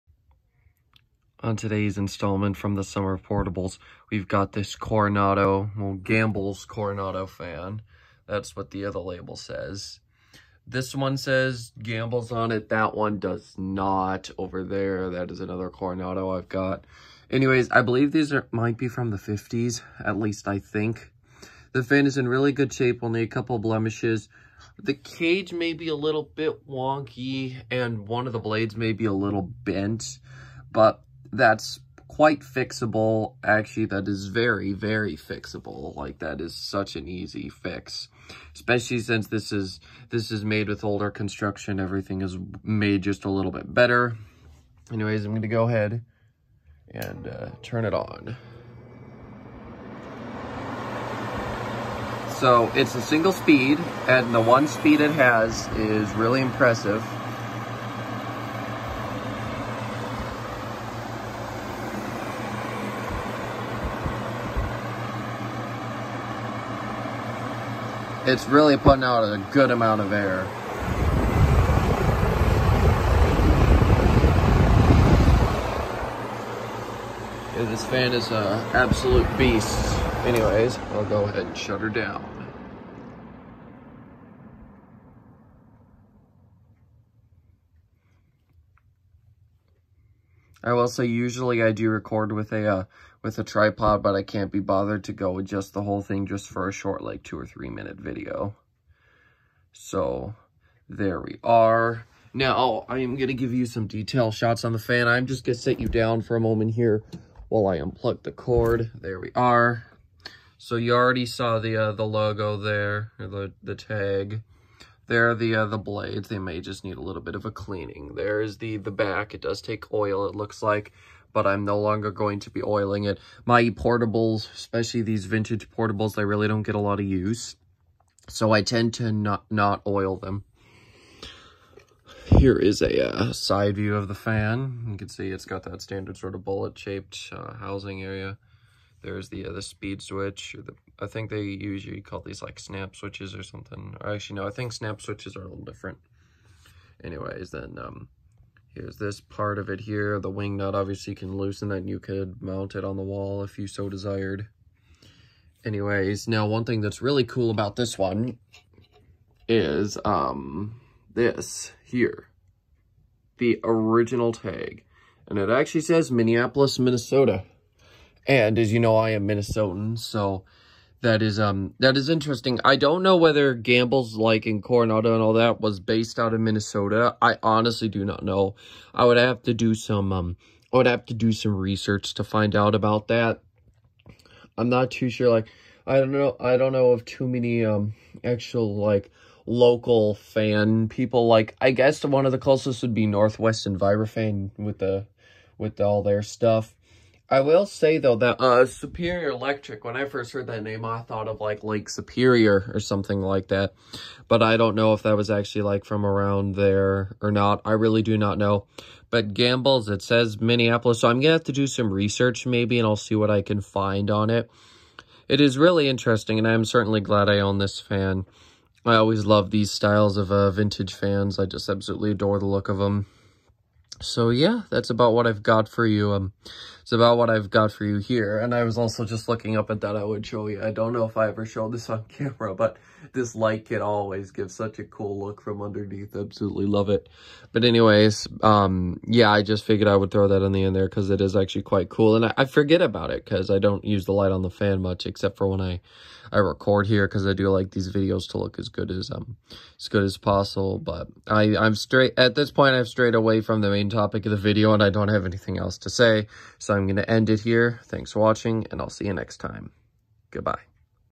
Vintage Gambles Coronado HW23-1045A table fan
Got this for 27$ at an antique store. Was the best deal I have had yet for a vintage portable.